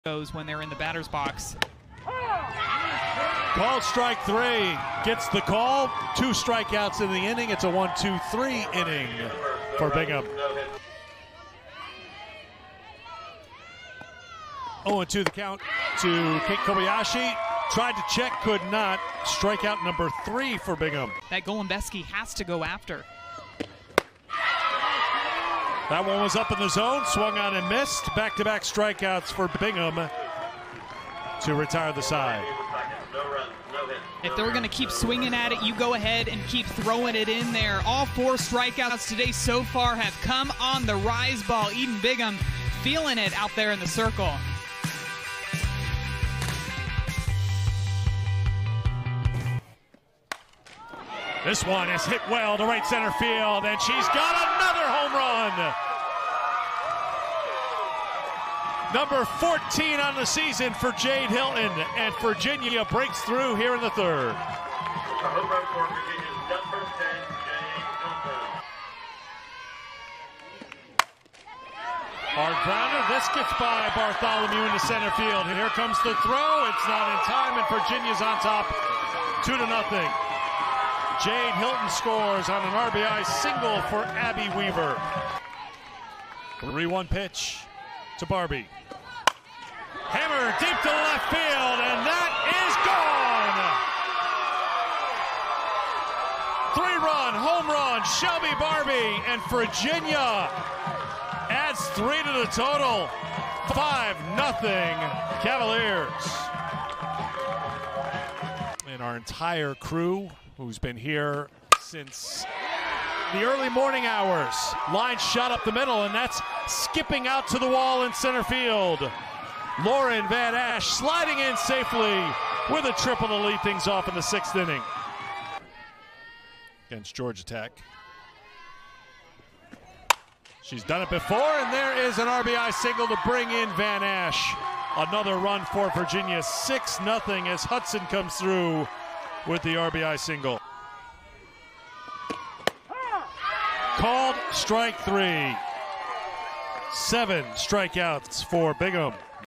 SOFTBALL - Knoxville Regional Game 3 Highlights
The Cavaliers shut out Miami to advance to the regional final.